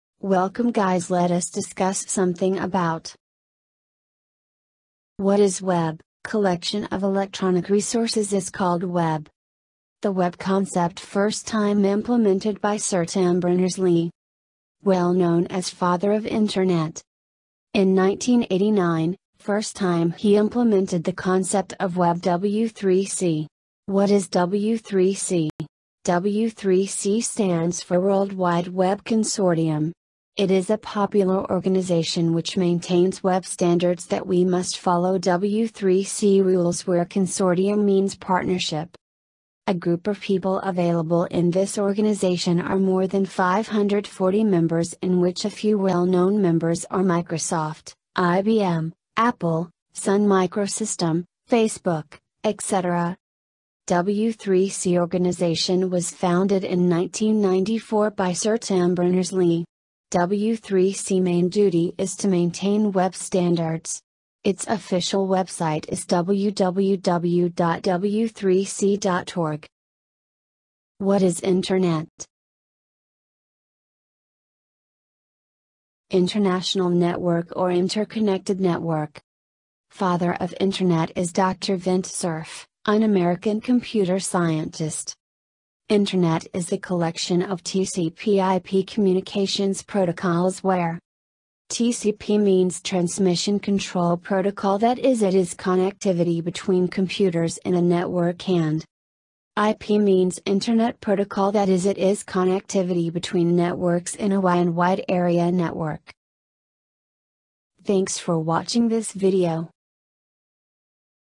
What is Web ( world wide web), W3C (world wide web consortium ), Internet , TCP , IP
Techoblasto  provides detail description of technology terminology or Information technology related terms  what is web or world wide web
what is w3c world wide web consortium
what is internet
what is TCP
What is IP